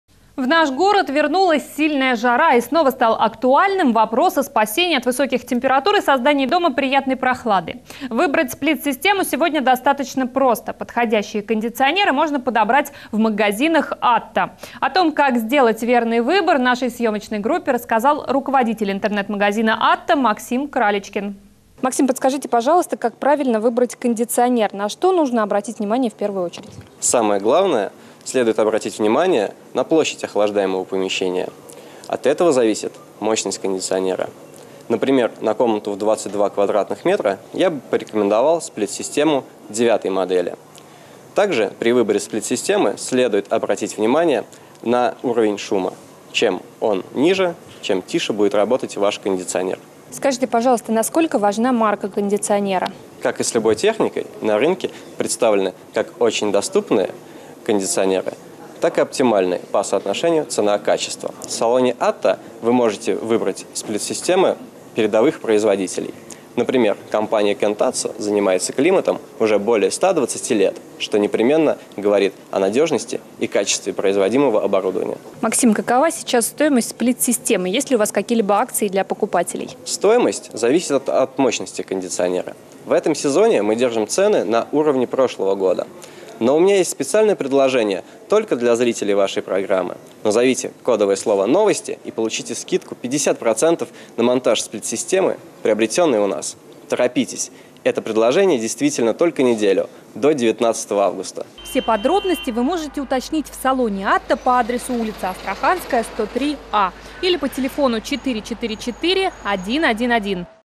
Магазин "АТТО"
Подходящие кондиционеры можно подобрать в магазинах "АТТО". О том, как сделать верный выбор, нашей съемочной группе рассказал руководитель интернет-магазина "АТТО" Максим Кралечкин.